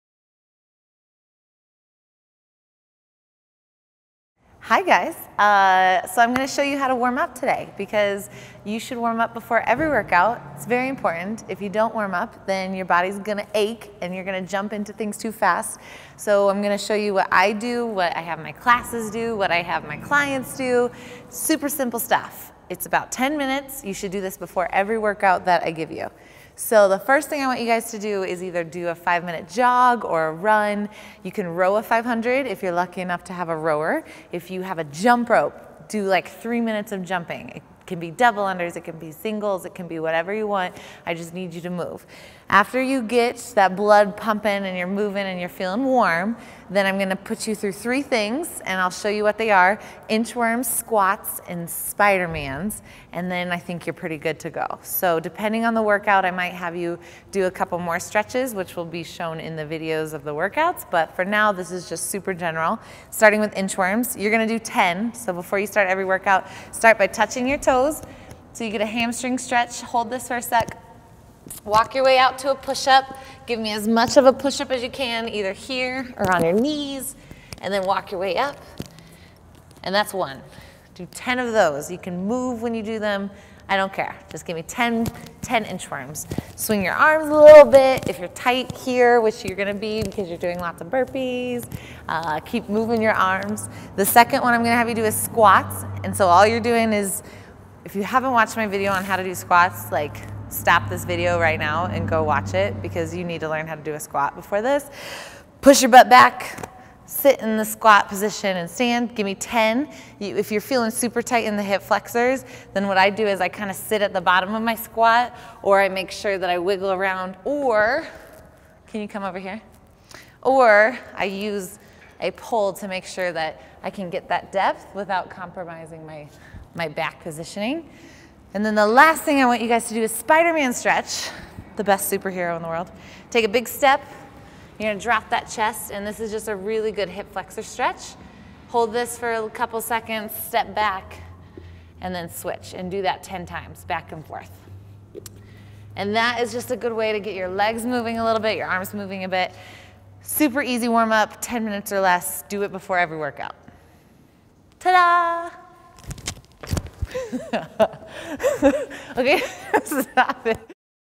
Warming Up for Your Workouts!
You have some pretty intense workouts about to happen in this 21 Day Superhero Challenge, 2.0 style. Don't be scurred, but BE PREPARED and move that body so that you are ready for it. 10 minutes or so is a great warmup time.